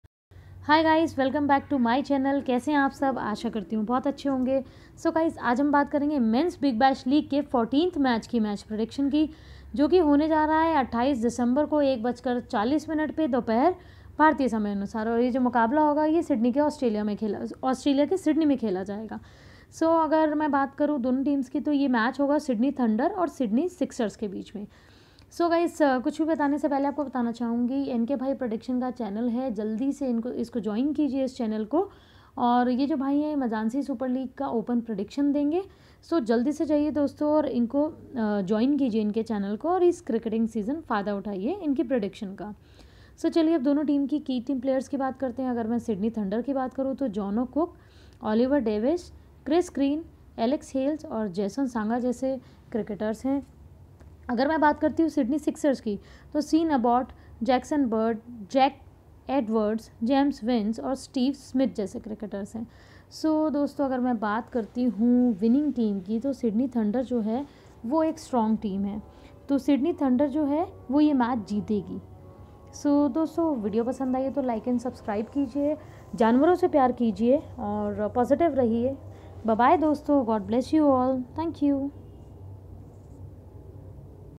Big Bash 2019-20 Sydney Sixers vs Sydney Thunder 14th Match Prediction | Dream 11 | SS vs ST
1. BBL 2019
2. 14th Match Preview
3. Sydney Sixers vs Sydney Thunder
4. Key Players
5. Dream 11
6. Pitch Report
8. T20 Records
9. Head To Head Match
10. Match Prediction